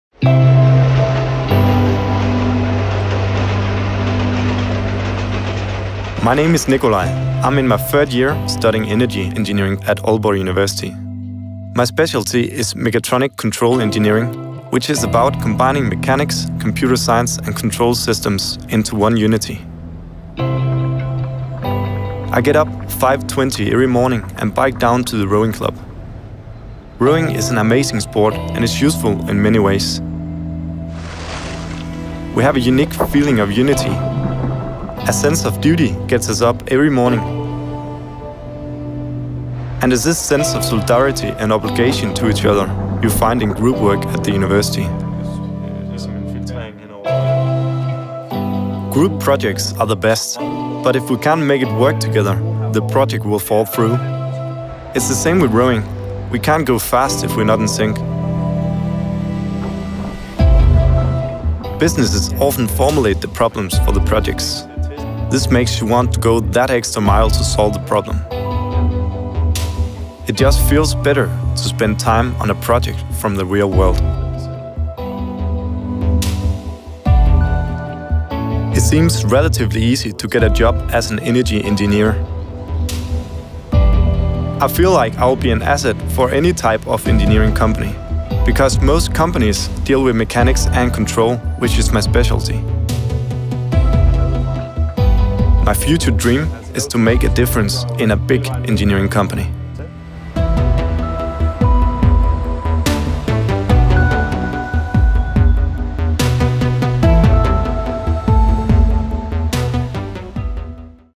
Study Energy Engineering at Aalborg University in Denmark
Meet Nikolaj who is studying Energy Engineering at Aalborg University in Denmark